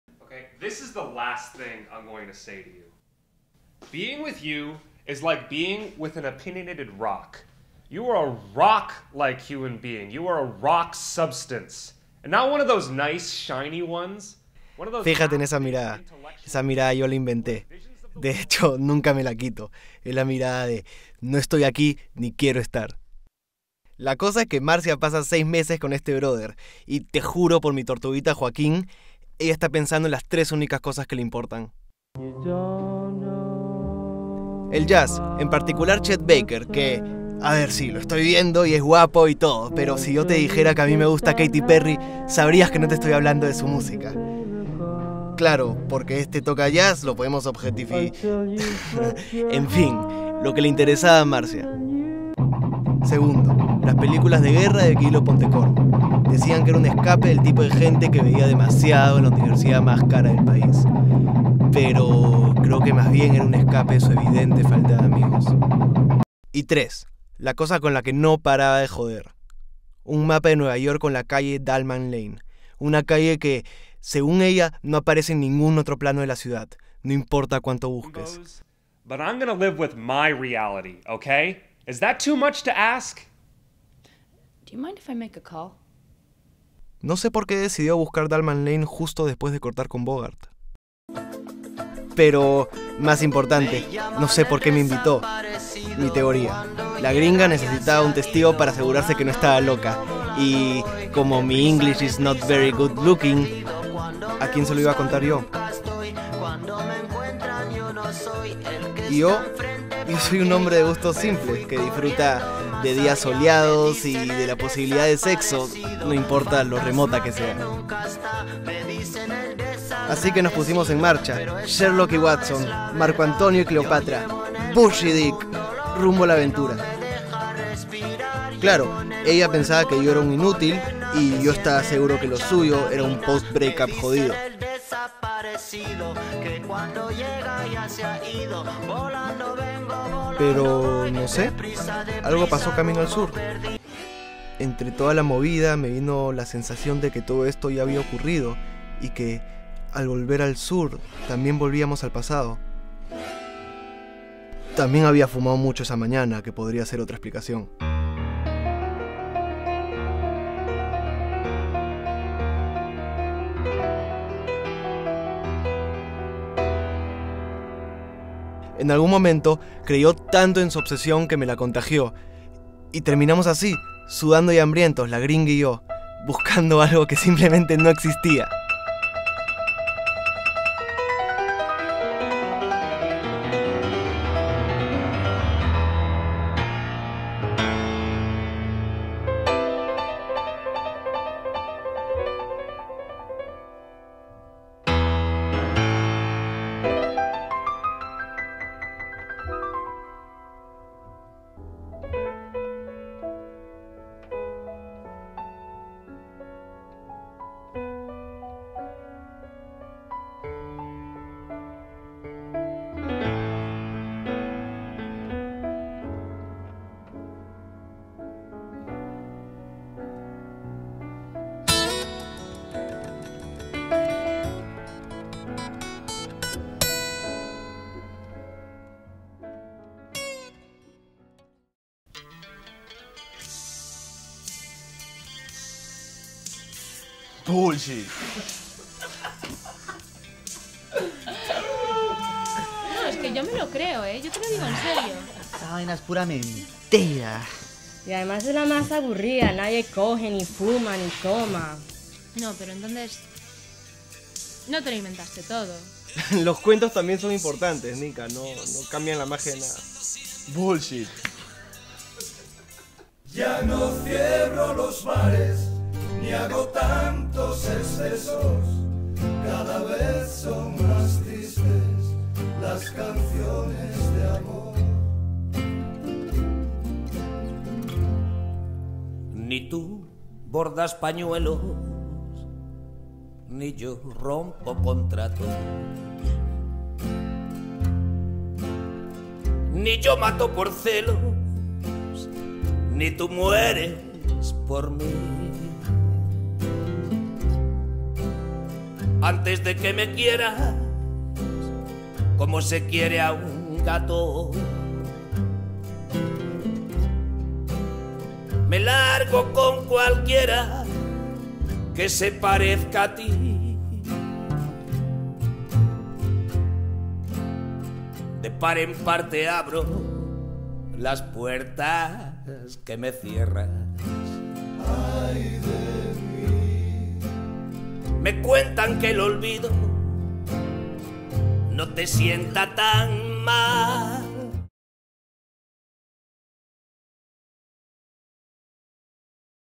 Dahlmann Lane
A gringa and a schlub search for a street that doesn't exist.

Final project for Sight & Sound Film, spring 2014.
NYU Tisch School of the Arts.